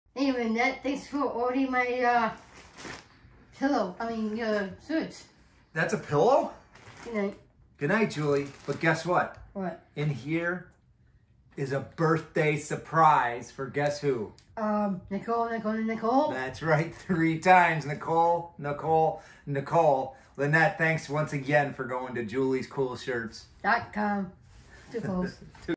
Order 1650 is headed to Nicole, Nicole, Nicole : )
Another envelope full of shirts and smiles is on it's way to one of ' s favorite friends. Our customers are the BEST! We truly love you Nicole and fam!